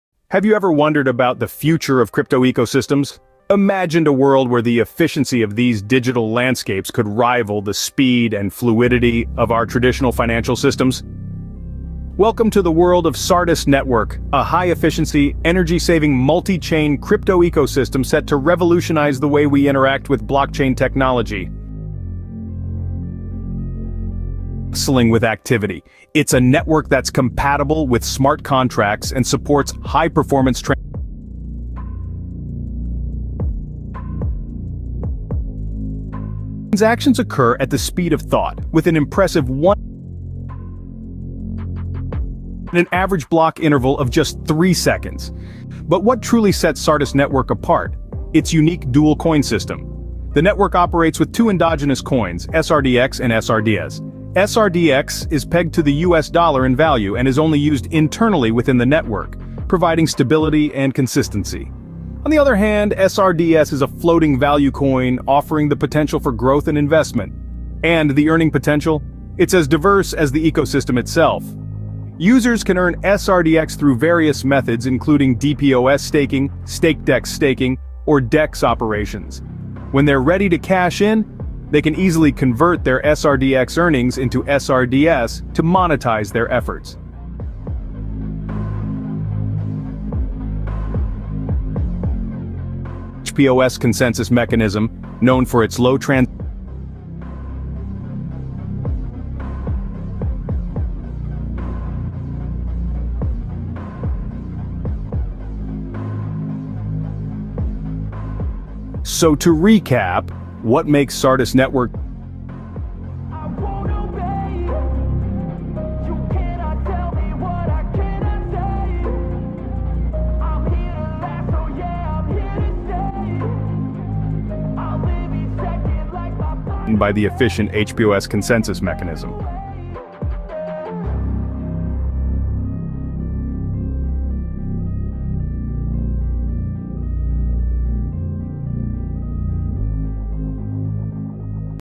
Introduction to Sardis Network 🌐
In this captivating video, we dive deep into the high-efficiency, energy-saving multichain crypto ecosystem of Sardis Network. Get ready to explore its unrivaled features and understand its tremendous potential in the world of cryptocurrency.

Sardis Network is revolutionizing the crypto industry with its cutting-edge technology and forward-thinking approach. By utilizing multiple chains, it ensures lightning-fast transactions, enhanced scalability, and minimal energy consumption. This innovative ecosystem opens up endless possibilities for decentralized finance (DeFi), global payments, and more.

OUTLINE:
00:00:00

00:00:21
The bustling activity of Sardis Network

00:00:45
The unique dual coin system of Sardis Network

00:01:07
The earning potential in Sardis Network

00:01:27
The consensus mechanism of Sardis Network

00:01:48
Recap of Sardis Network's standout features

00:02:09
Conclusion and invitation to the future of cryptocurrency